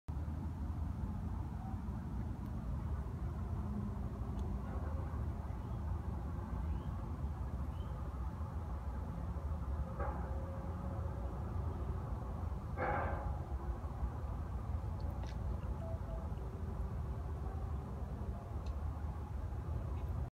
Are Golden Retrievers Suitable for Warm Climate?
How do Goldens like hot weather? As long as they have access to shade, free moving air, and water, they will do just fine ...